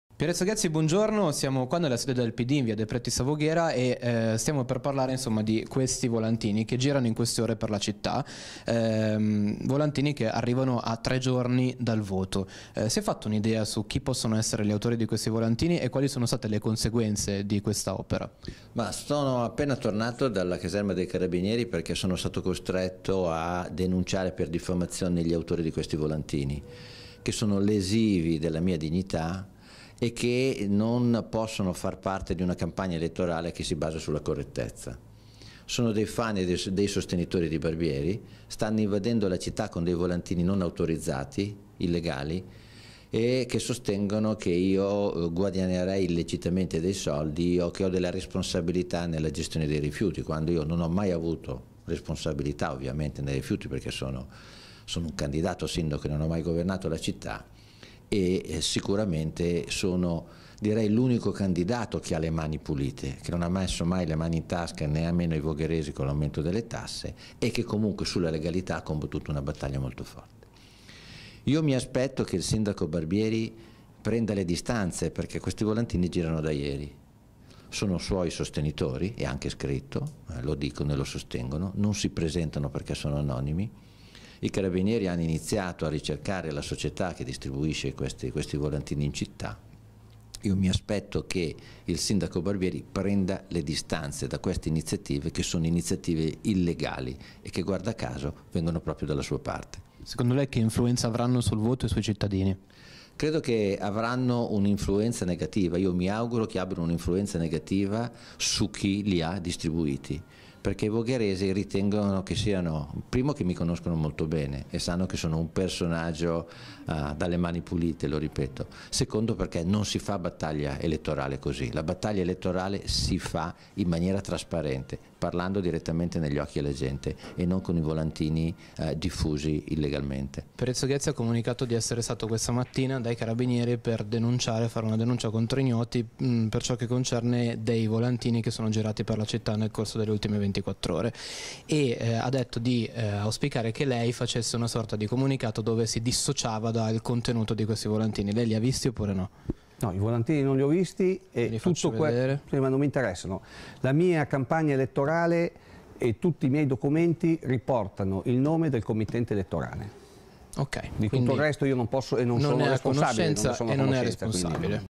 Ghezzi dai carabinieri: “volantini diffamanti dal centrodestra”. Barbieri: “non ne so nulla”
Produzione: Jab Media